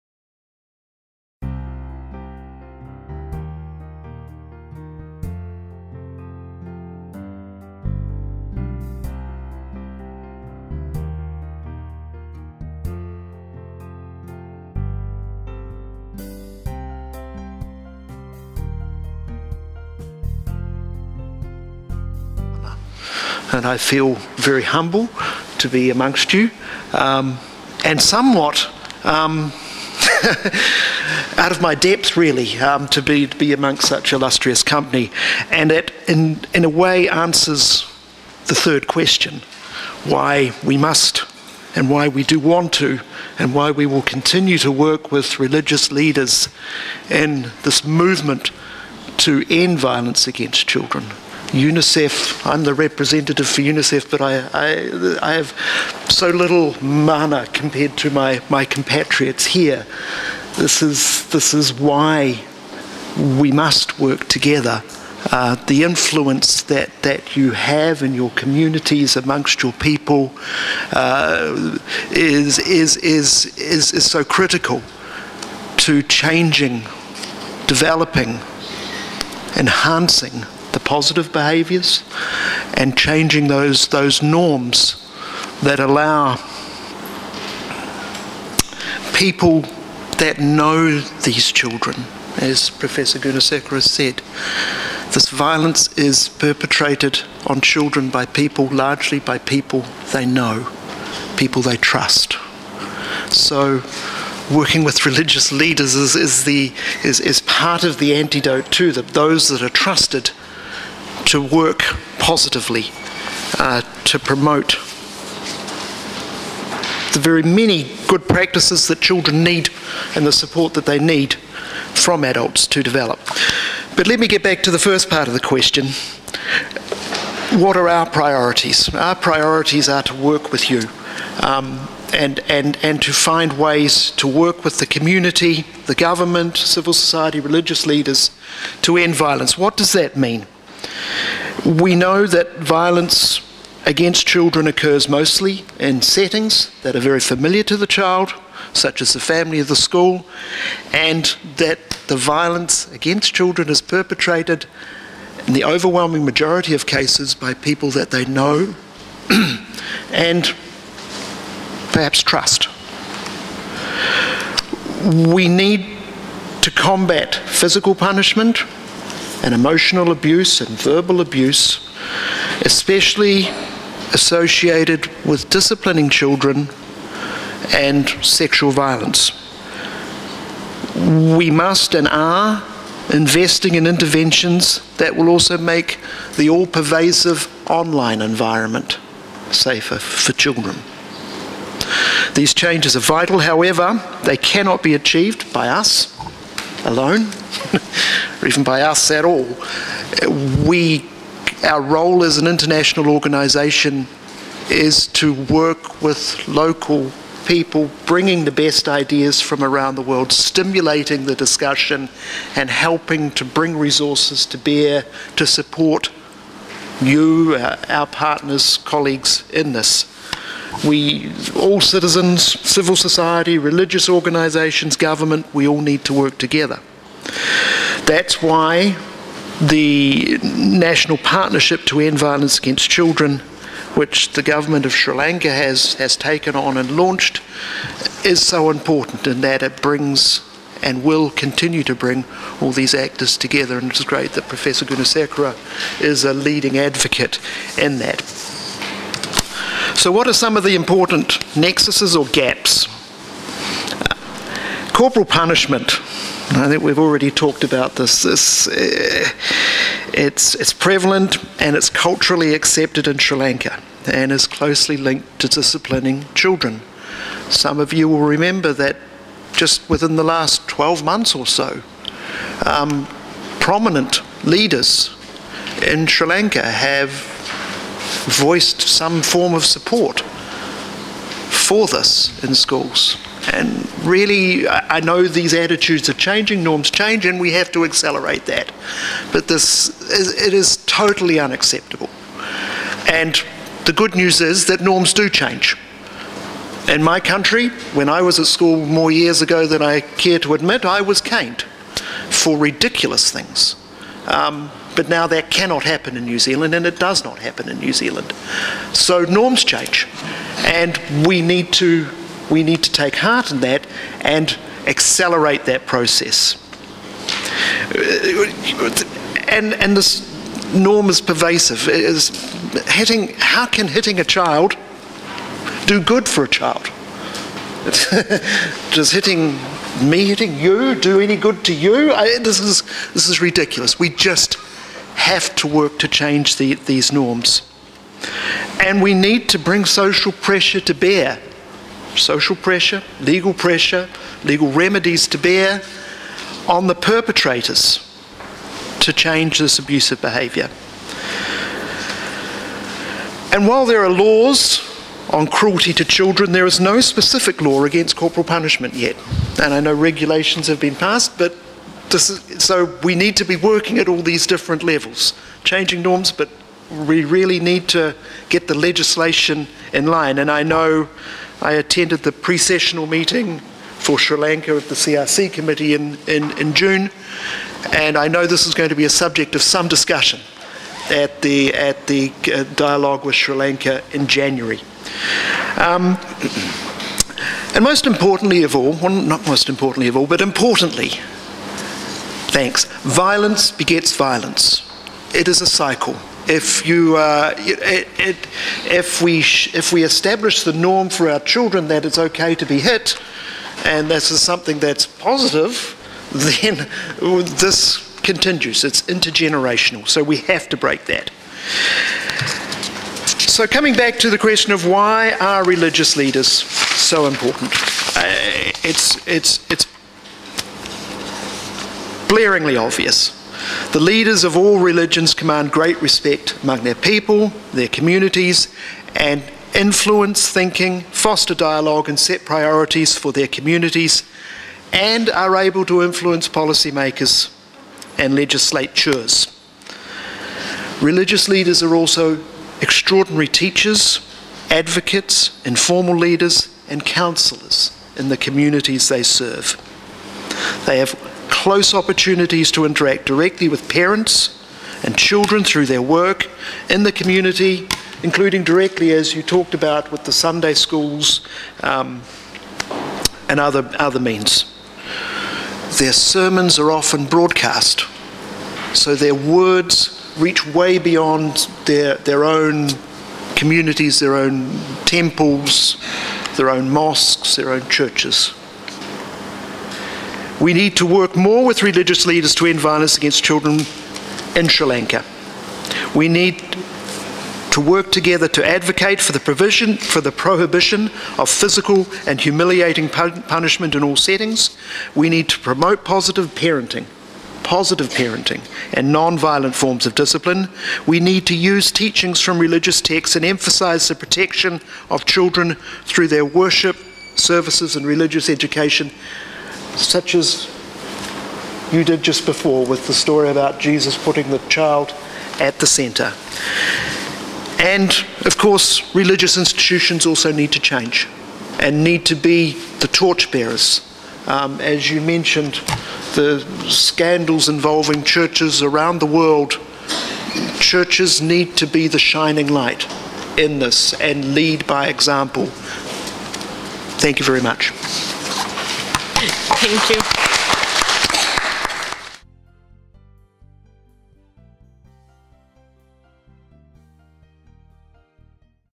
5- Mr. Tim Sutton
Ethics Education to Strengthen Families and Nurturing Spirituality in Early Childhood for the Prevention of Violence Against Children:
The Role of Religious Communities

Roundtable Discussion
23 August 2017 – 15:30 to 19:00
Sri Lanka

Co-organized by Sarvodaya, Walpola Rahula Institute, the Global Partnership to End Violence Against Children and Arigatou International